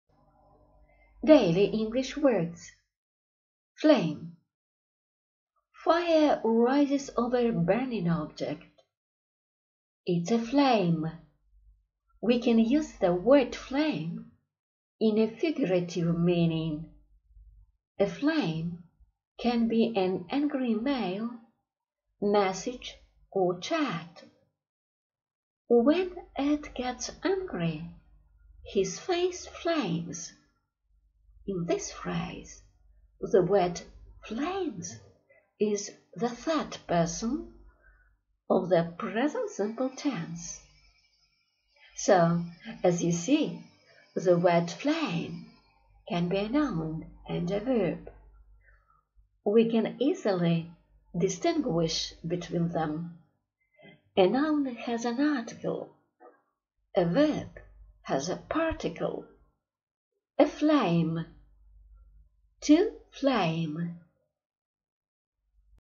💜💙💜 Daily English Words 💦💦💦35 FLAME 👍 2020
35. FLAME. Fire rises over a burning object. It's a flame. We can use the word “flame” in a figurative meaning.
A flame can be  an angry email, message, or chat.
When Ed gets angry, his face flames. In this phrase the word FLAMES is the third person of the present simple tense. So as you see the word flame can be a noun and a verb. We can easily distinguish between them. A noun has an article, a verb has a particle. A flame - to flame.